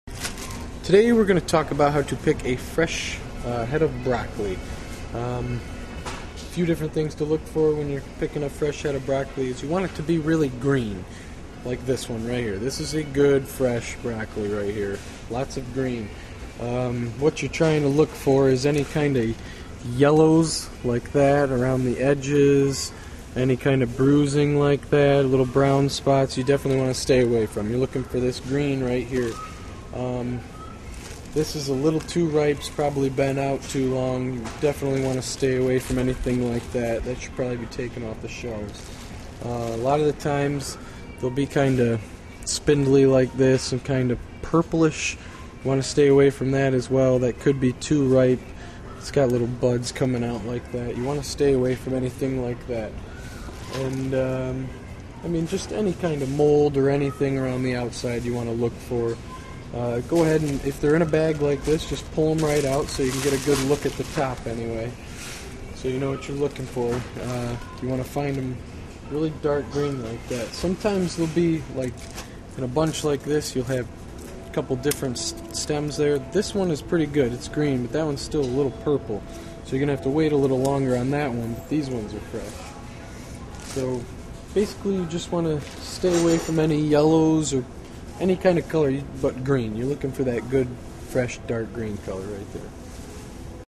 How to Pick Out Broccoli - 2008-07-12
Fresh broccoli should be really green and should not have any yellow or bruising. Learn about picking out broccoli at a grocery store or supermarket in this free video on fresh produce and vegetables. Published on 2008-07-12